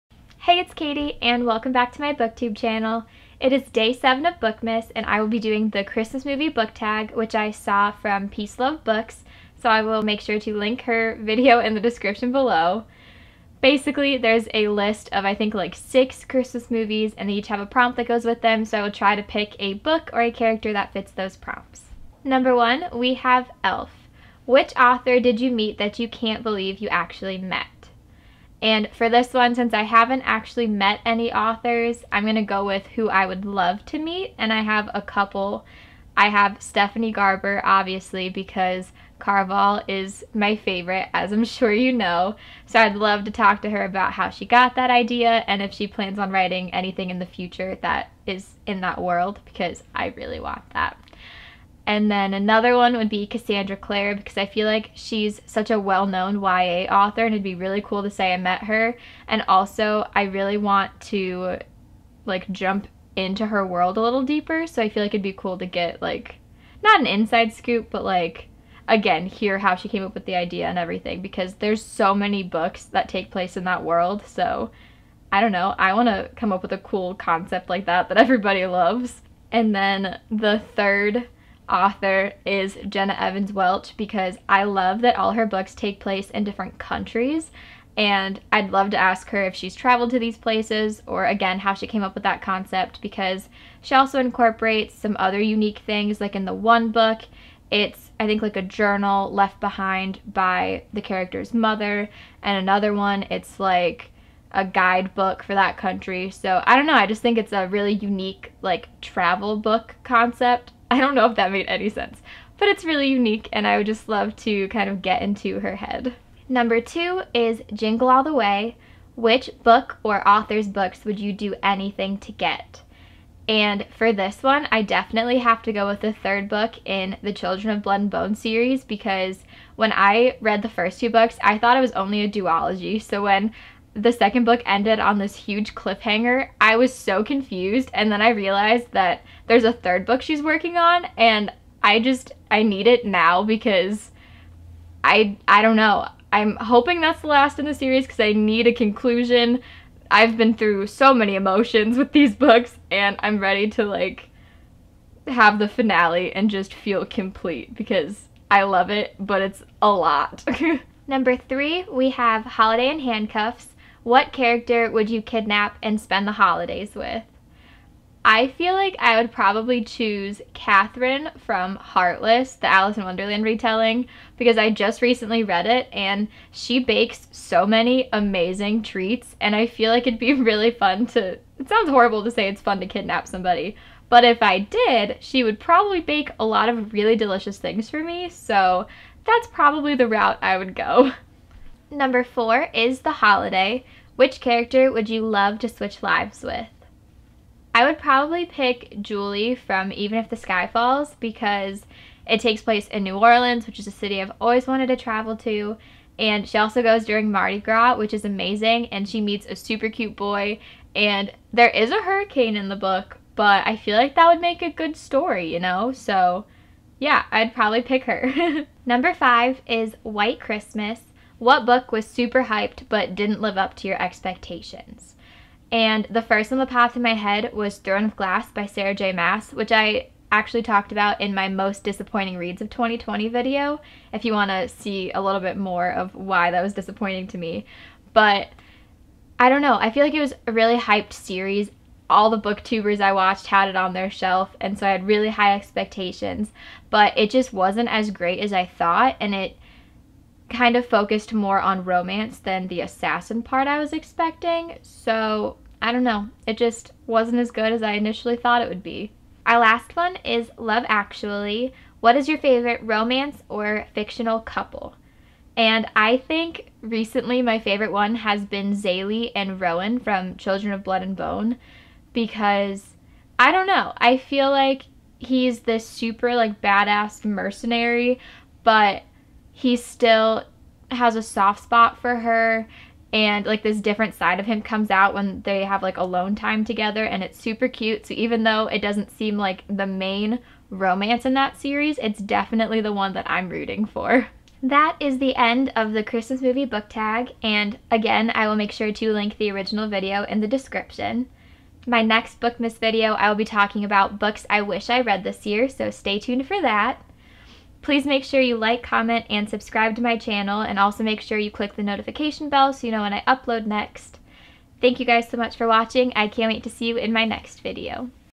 Christmas Movie Book Tag | Bookmas 2020
Hey everyone! Today I'm doing the Christmas Movie Book Tag. Be sure to check out PeaceLoveBooks' original video. Enjoy!